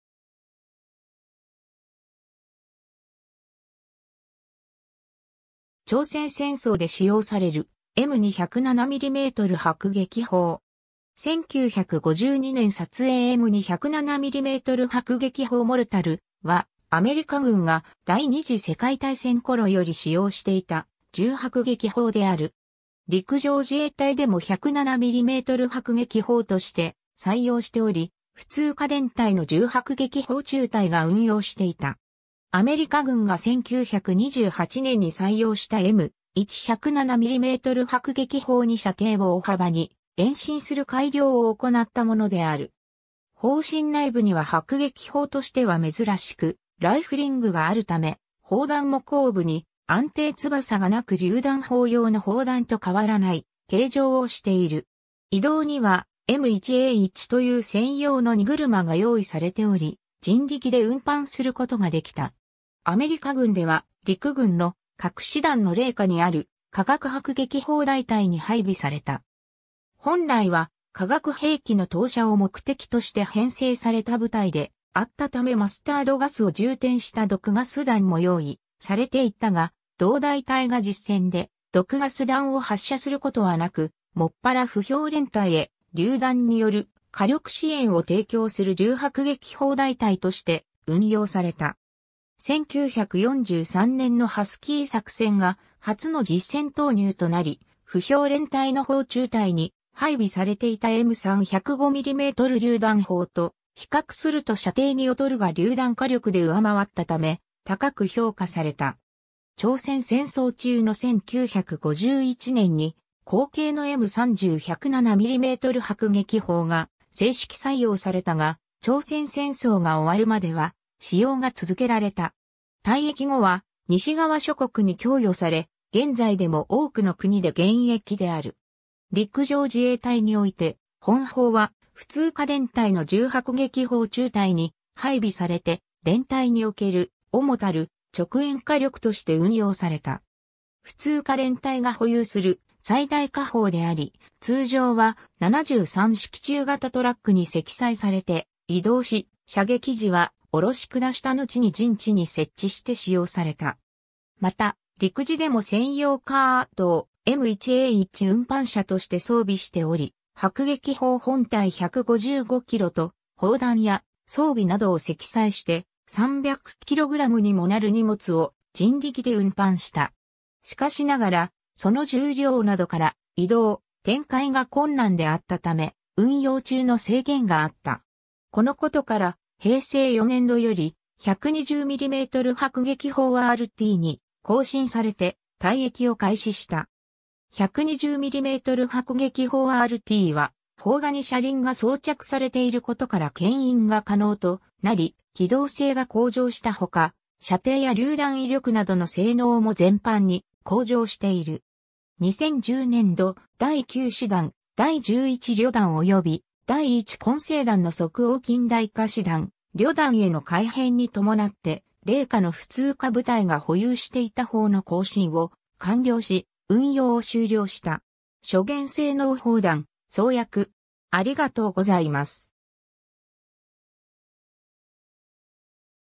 M2 107mm迫撃砲
朝鮮戦争で使用されるM2 107mm迫撃砲。
1952年撮影 M2 107mm迫撃砲（エム2 107ミリはくげきほう、M2 4.2"（107mm）Mortar）は、アメリカ軍が第二次世界大戦頃より使用していた重迫撃砲である。
 陸上自衛隊でも107mm迫撃砲（初期には4.2インチ迫撃砲とも）として採用しており、普通科連隊の重迫撃砲中隊が運用していた。
 アメリカ軍が1928年に採用したM1 107mm迫撃砲に射程を大幅に延伸する改良を行ったものである。
砲身内部には迫撃砲としては珍しくライフリングがあるため、砲弾も後部に安定翼が無く榴弾砲用の砲弾と変わらない形状をしている。
移動にはM1A1という専用の荷車が用意されており、人力で運搬する事ができた。
 アメリカ軍では陸軍の各師団の隷下にある化学迫撃砲大隊に配備された。
本来は化学兵器の投射を目的として編成された部隊であったためマスタードガスを充填した毒ガス弾も用意されていたが、同大隊が実戦で毒ガス弾を発射することは無く、専ら歩兵連隊（連隊戦闘団）へ榴弾による火力支援を提供する重迫撃砲大隊として運用された。
 1943年のハスキー作戦（シチリア上陸作戦）が初の実戦投入となり、歩兵連隊の砲中隊に配備されていたM3 105mm榴弾砲と比較すると射程に劣るが榴弾火力で上回ったため高く評価された。
朝鮮戦争中の1951年に後継のM30 107mm迫撃砲が制式採用されたが、朝鮮戦争が終わるまでは使用が続けられた。
退役後は西側諸国に供与され、現在でも多くの国で現役である。
 陸上自衛隊において本砲は、普通科連隊の重迫撃砲中隊に配備されて、連隊における主たる直掩火力として運用された。
普通科連隊が保有する最大火砲であり、通常は、73式中型トラックに積載されて移動し、射撃時は卸下したのちに陣地に設置して使用された。
また、陸自でも専用カートをM1A1運搬車として装備しており、迫撃砲本体155キロと砲弾や装備などを積載して、300キログラムにもなる荷物を人力で運搬した。
しかしながら、その重量などから移動・展開が困難であったため、運用中の制限があった。
 このことから、平成4年（1992年）度より、120mm迫撃砲 RTに更新されて退役を開始した。
120mm迫撃砲 RTは、砲架に車輪が装着されていることから牽引が可能となり、機動性が向上したほか、射程や榴弾威力などの性能も全般に向上している。
2010年度、第9師団、第11旅団および第1混成団の即応近代化師団/旅団への改編に伴って、隷下の普通科部隊が保有していた砲の更新を完了し、運用を終了した。
 諸元 性能 砲弾・装薬